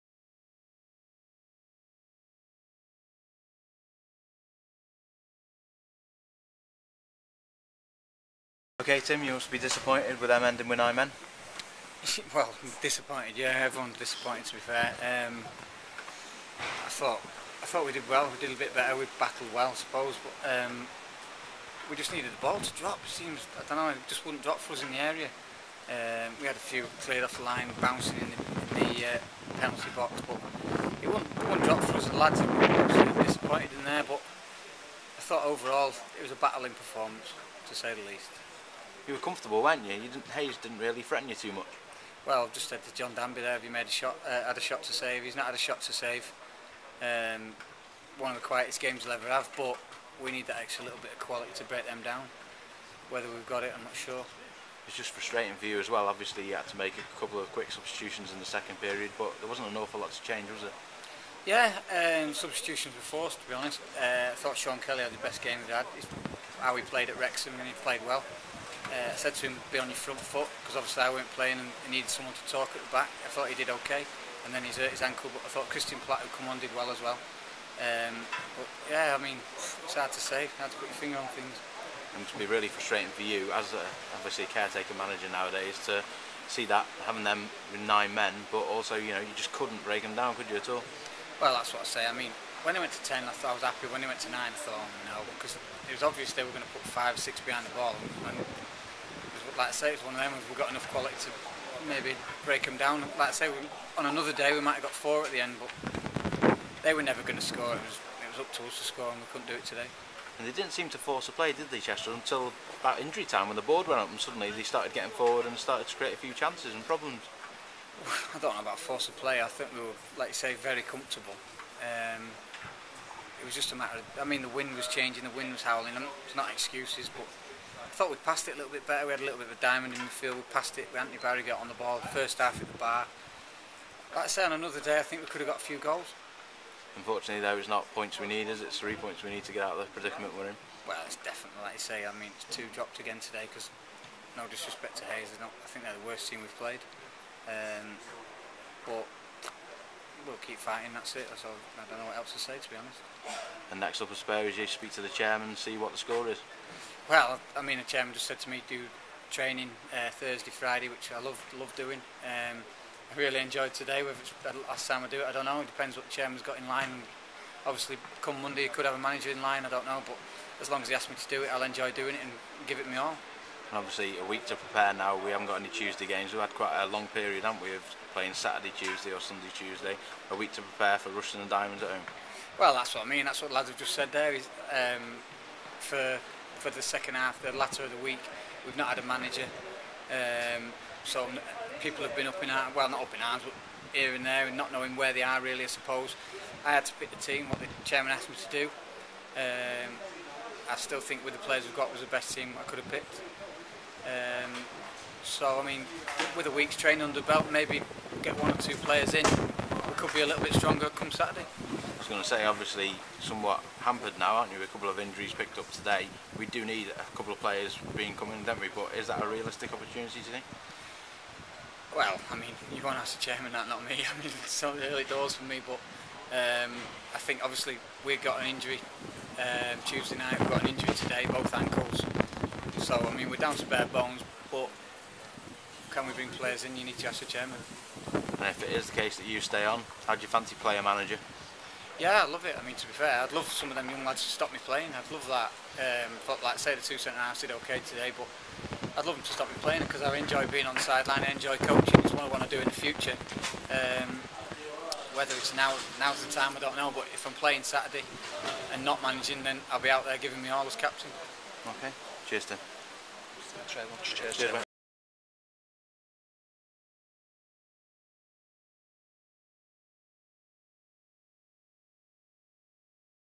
Hayes & Yeading v Chester City post-match interview
Chester City caretaker manager/captian Tim Ryan talks to us after his first game in charge. He speaks about the future of Chester and his hopes for management.